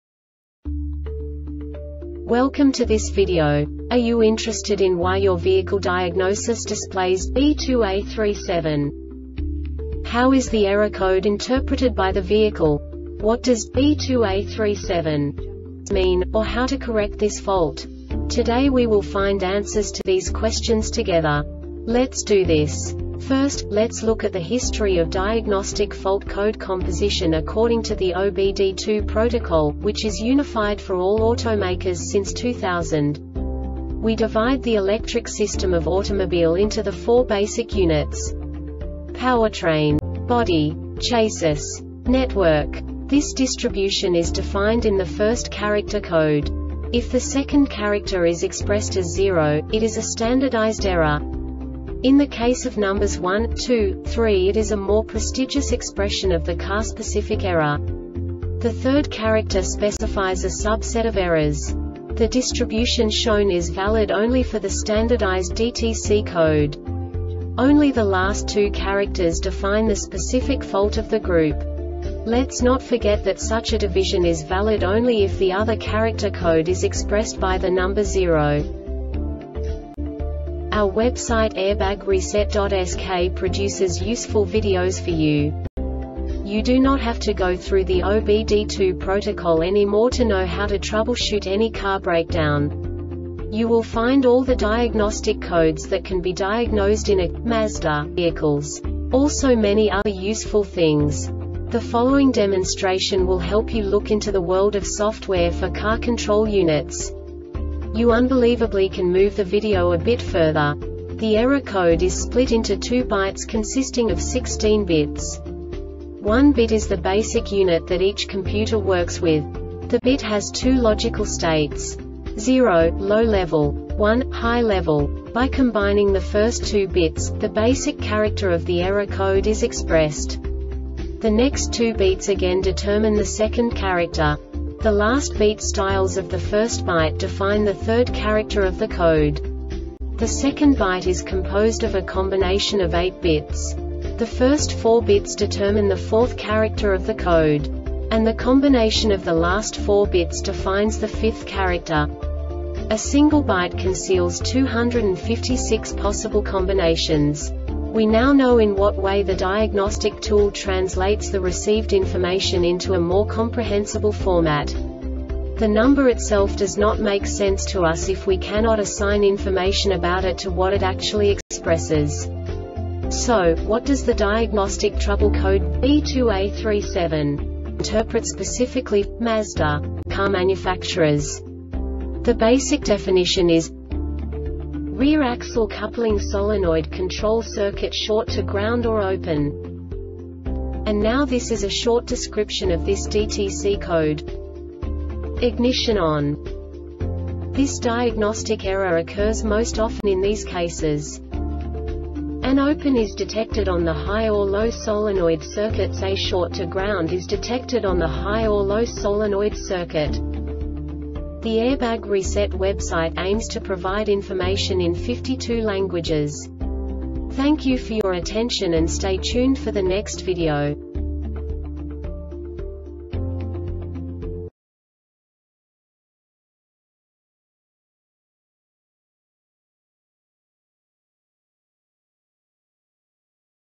DTC Mazda B2A37 Short Explanation
Contents:
0:21 Basic DTC analysis according to OBD2 protocol standard.
1:48 Insight into programming
2:58 A diagnostically understandable description of the Diagnostic Trouble Code
3:05 Basic error definition

Make:
Mazda
Code:
B2A37
Definition:
Rear Axle Coupling Solenoid Control Circuit Short to Ground or Open
Description:
Ignition ON.
Cause:
An open is detected on the high or low solenoid circuits.A short to ground is detected on the high or low solenoid circuit.